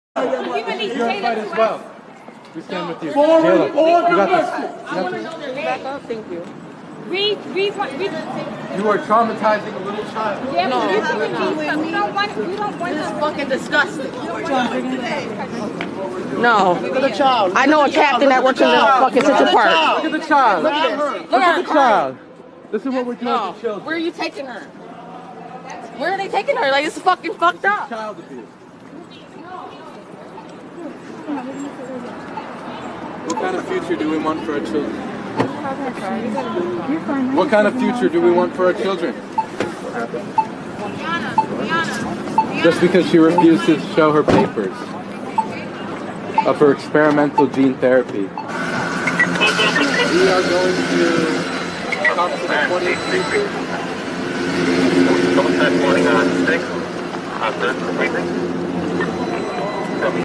NYPD arrested a 9 year old child in NYC because she didn’t have a vax card in a museum.
NYPD arrested a 9 year old child in NYC because she didn’t have a vax card in a museum. We are destroying lives. This has to stop! gustapo in full effect. German nazis. SS